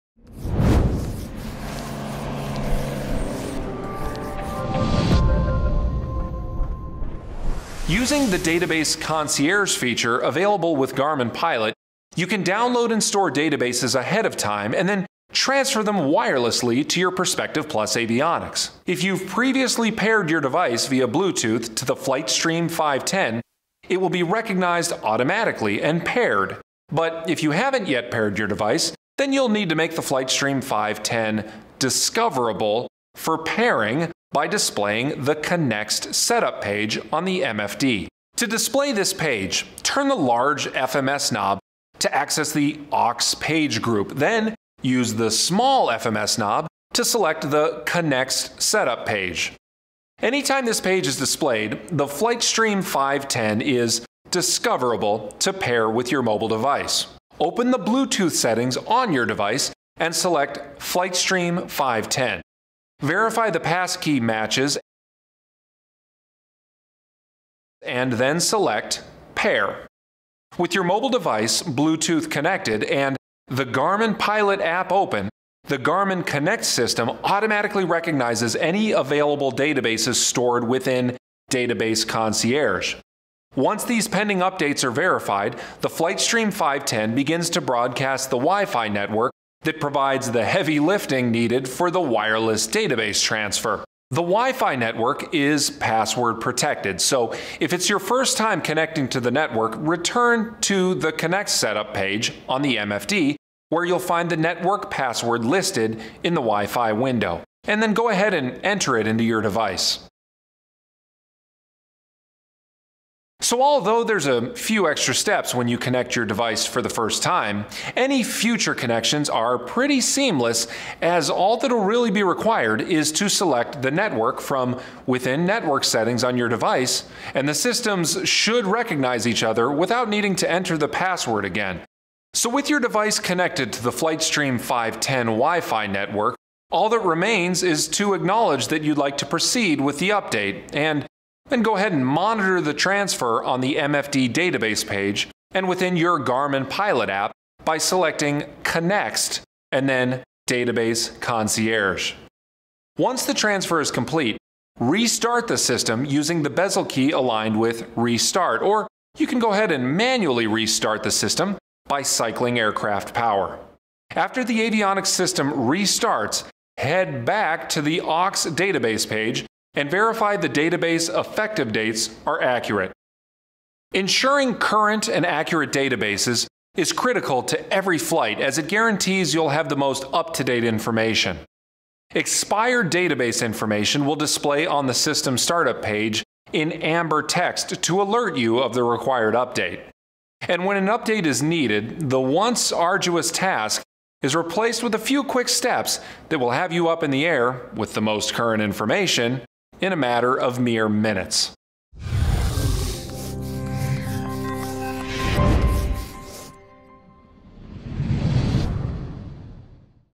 Wireless Updating | Perspective+ Avionics
Using the Garmin Pilot app, database updates can be done wirelessly for the Perspective+ by Garmin avionics. This video shows the steps.